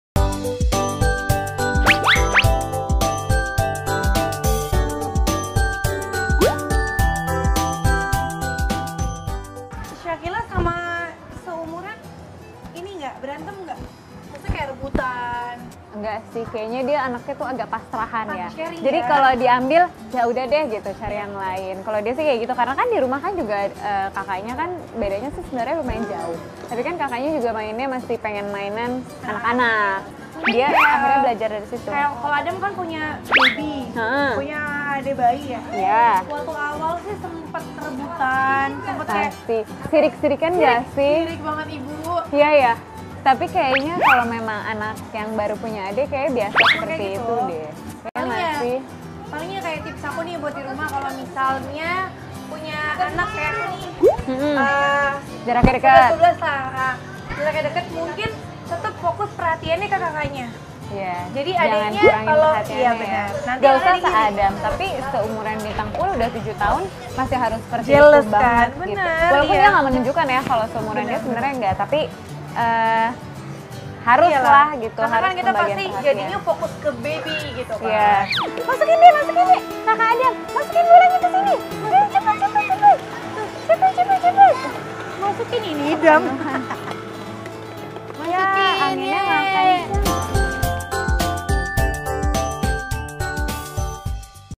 Cara Shireen Sungkar dan Sonya Fatmala Agar Kakak-Adik Akur
Amit-amit jangan sampai si kakak dan adiknya nggak akur alias ada sibling rivalry di antara mereka. Nah, Shiren Sungkar dan Sonya Fatmala punya cara sendiri untuk mencegahnya lho.